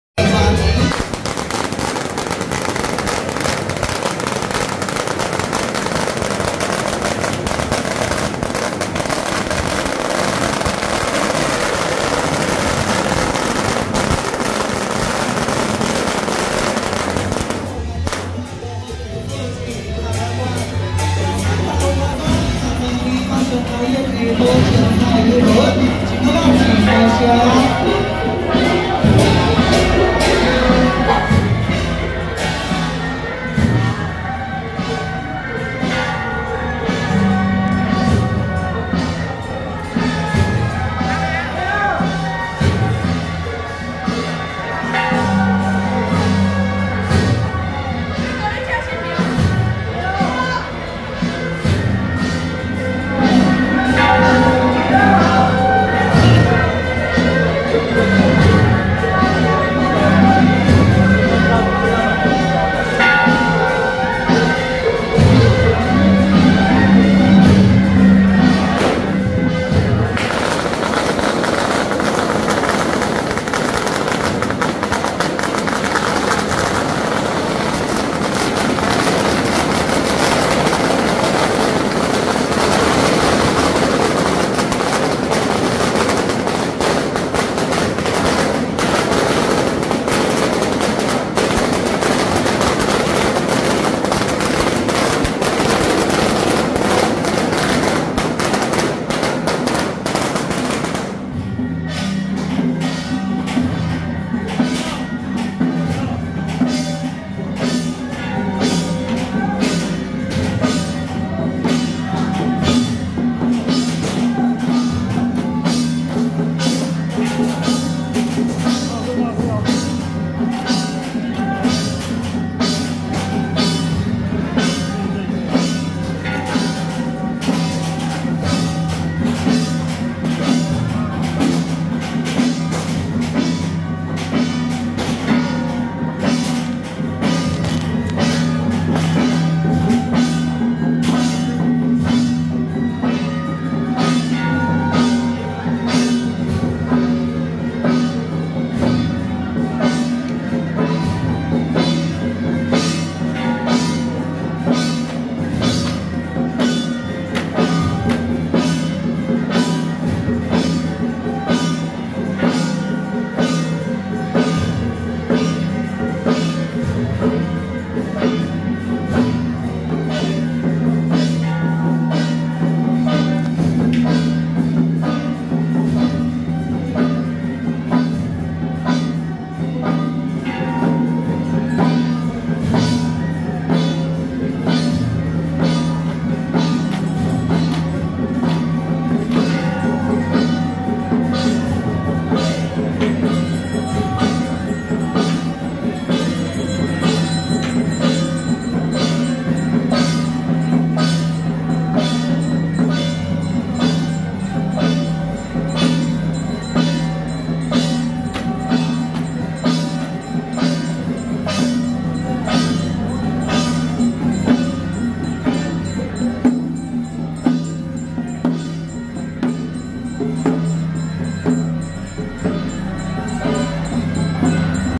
【淡水油車口忠義宮-蘇府王爺 聖駕】~112淡水油車口忠義宮恭祝蘇府王爺聖誕遶境
淡水油車口忠義宮-蘇府王爺 聖駕